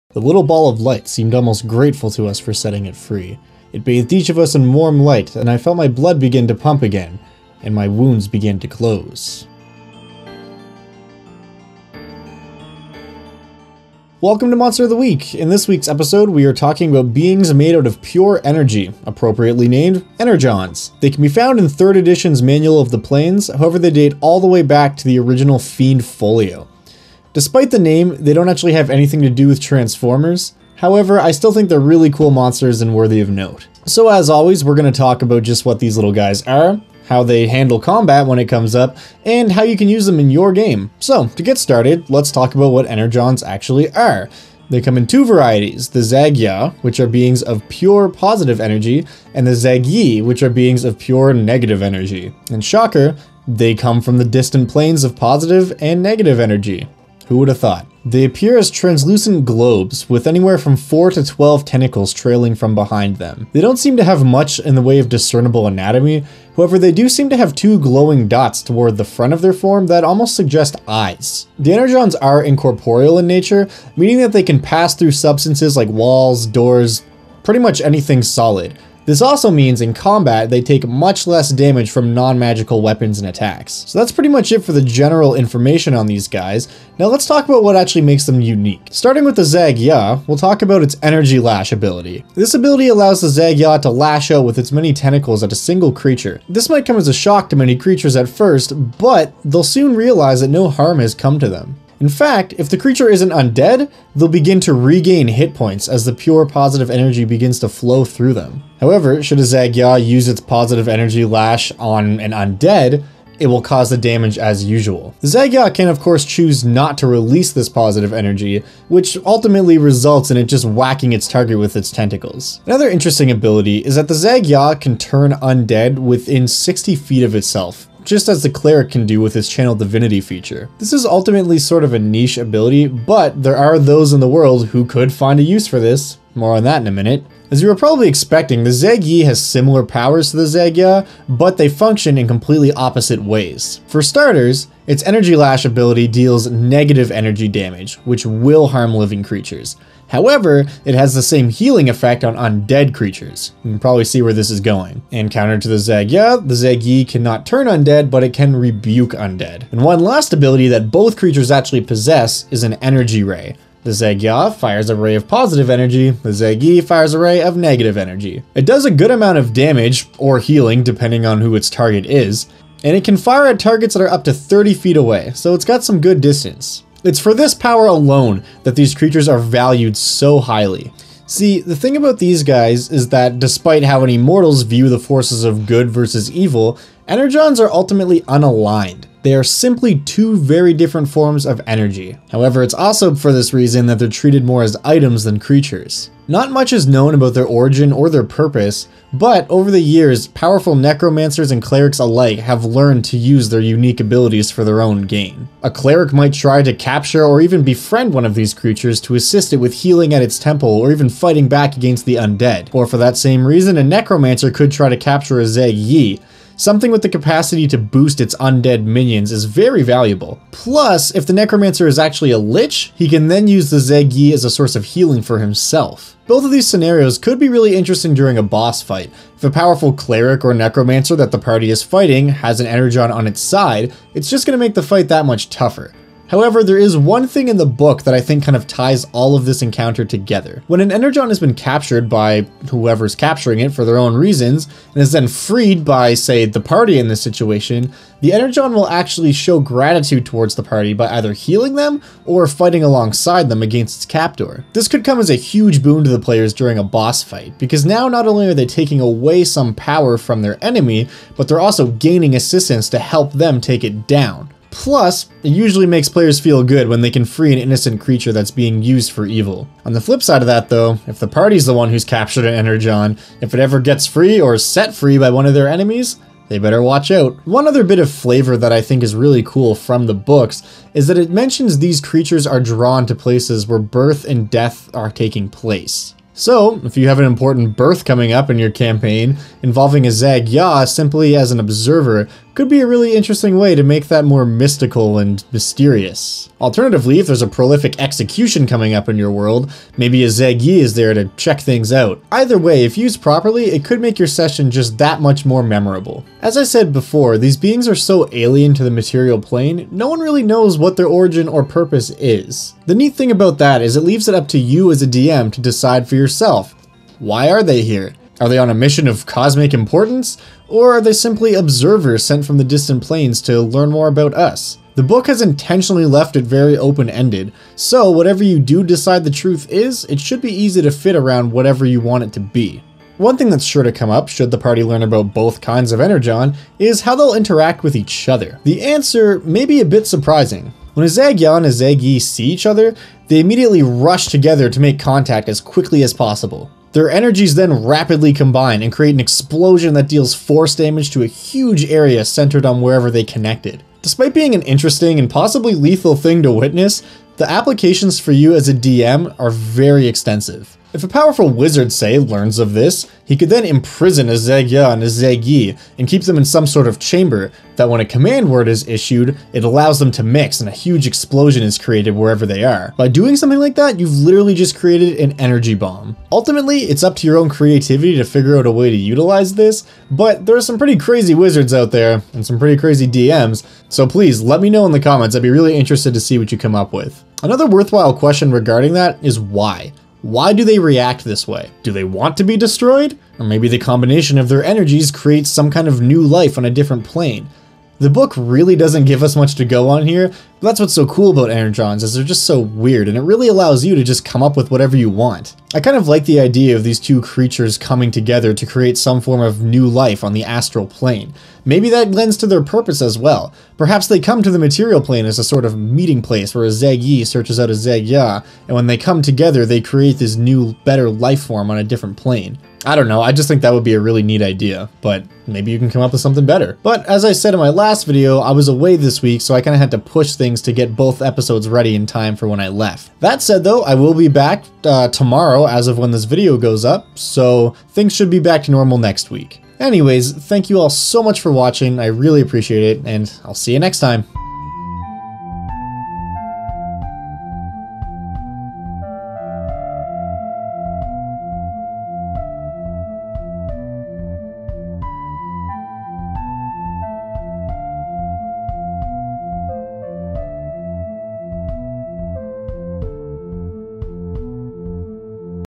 Monster of the Week - Energons - Dungeons & Dragons [D&D]
This week, we take a look at Energons!

shout out to Table Top Audio for the fantastic background music